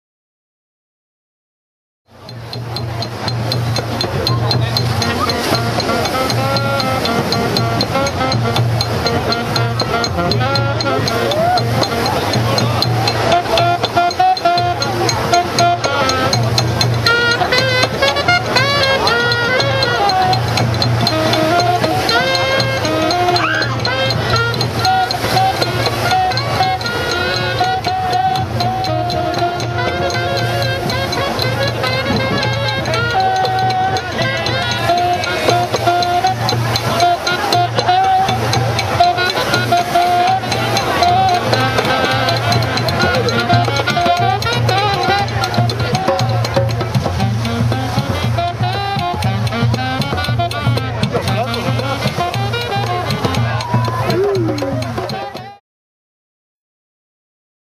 firedance~love earth, feel the fire~
firedance performance in Japan @ the beach party